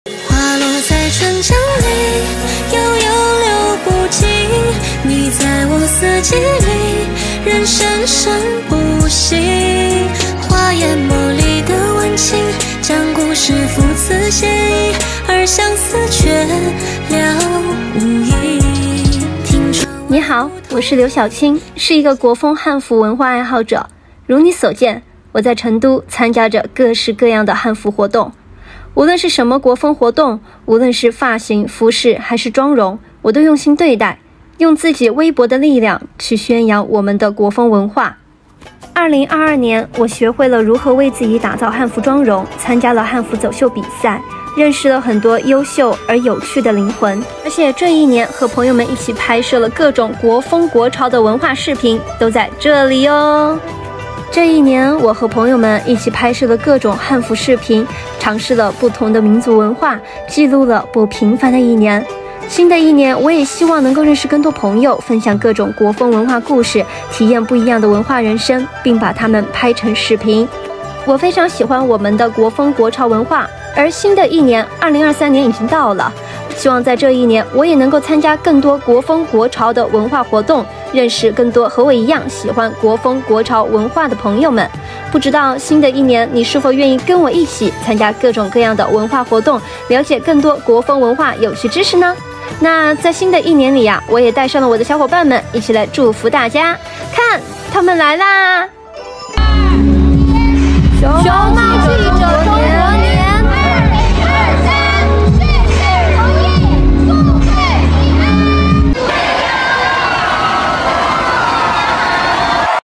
In 2023, come and see how Liu Xiaoqing, an enthusiast of Chinese traditional clothing culture
In 2023, come and see how Liu Xiaoqing, an enthusiast of Chinese traditional clothing culture, uses her modest strength to promote our national culture.